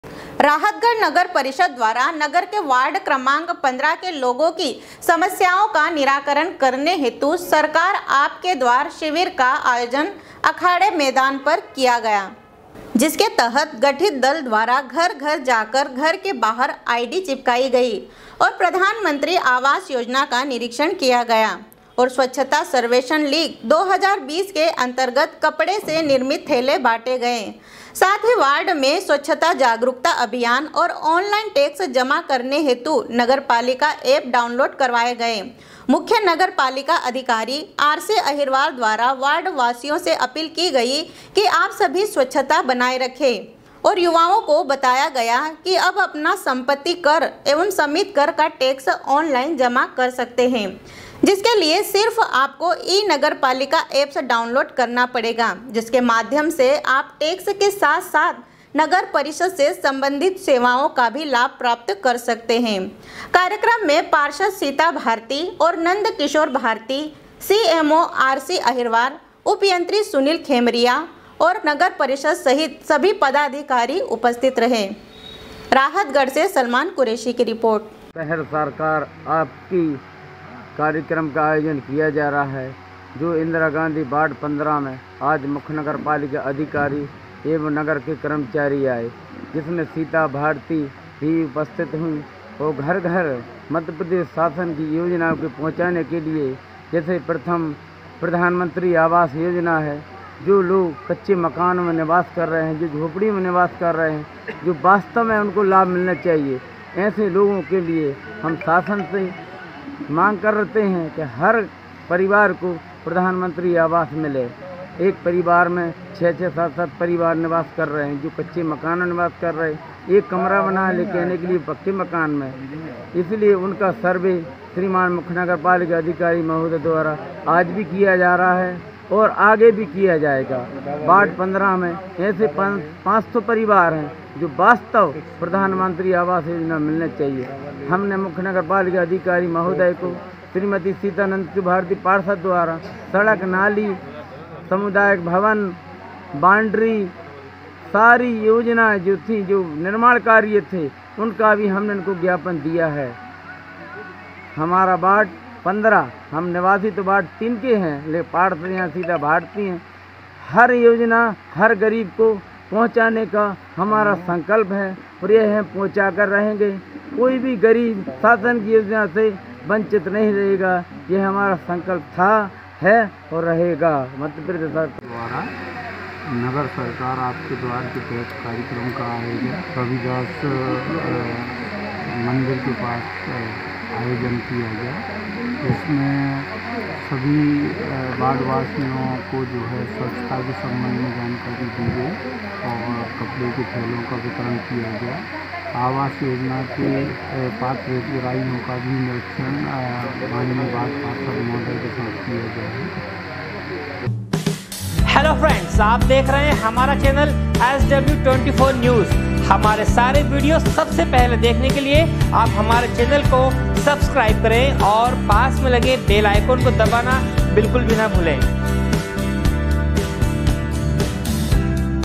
sw24news rahtgarh, 22.10.2019
राहतगढ़ नगर परिषद द्वारा नगर के वार्ड क्रमांक 15 के लोगों की समस्याओं का निराकरण करने हेतु सरकार आपके द्वार शिविर का आयोजन अखाड़े मैदान पर किया गयाsw24news